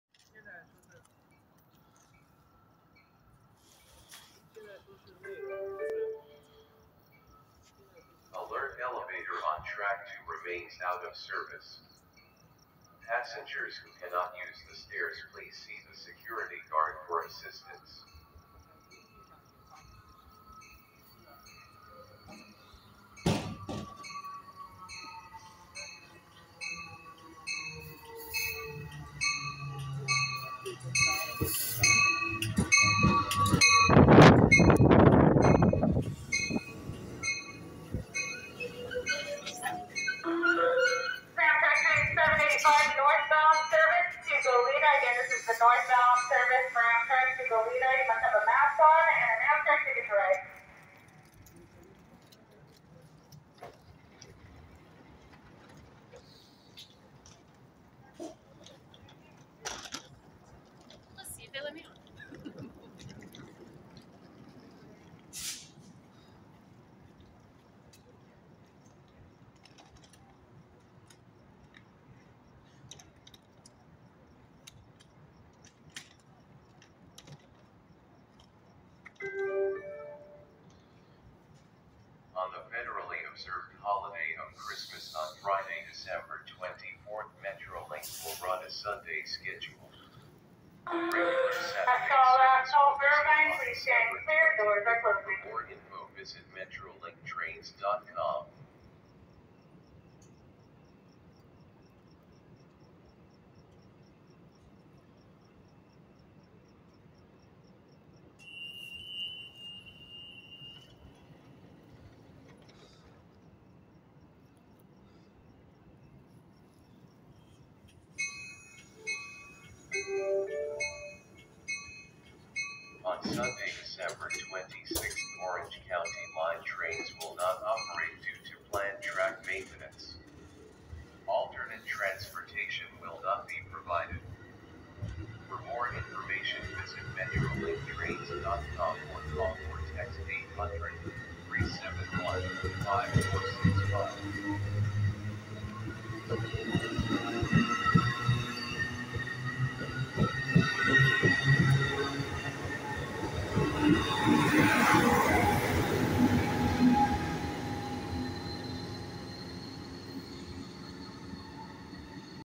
Amtrak 785 @ Irvine
Amtrak train 785 with CDTX 6953 leading and CDTX 2113 pushing, arrives and departs Irvine, CA. Filmed 12/21/2021.